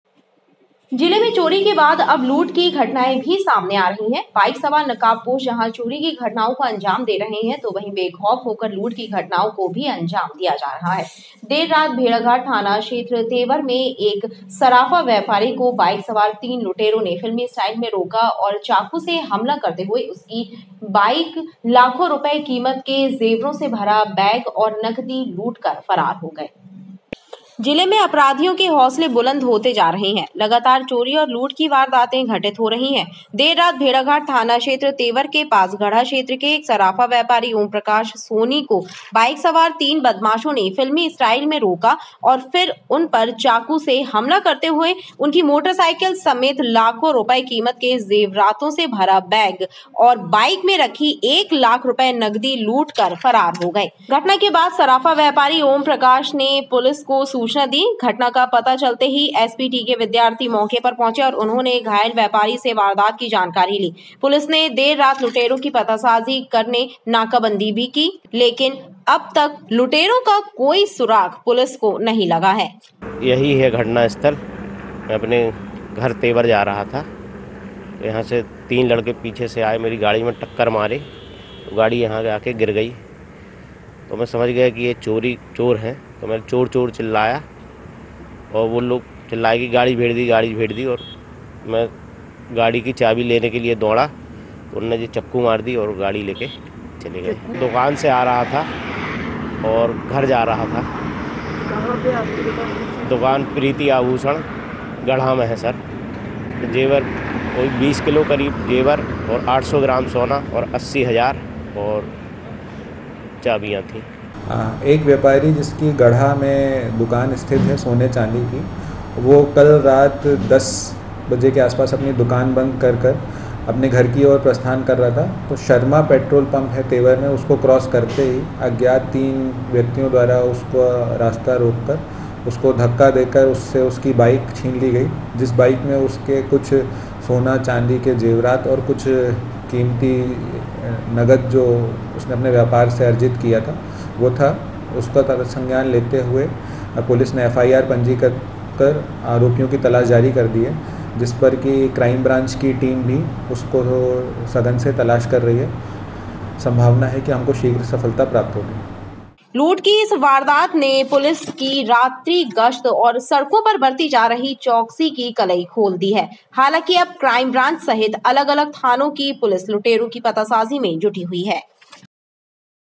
जबलपुर - तेवर के पास सराफा व्यापारी को चाकू मारकर लूटा, बाइक सवार लुटेरों ने दिया वारदात को अंजाम...
देर रात घटना स्थल पर पहुंचे पुलिस अधीक्षक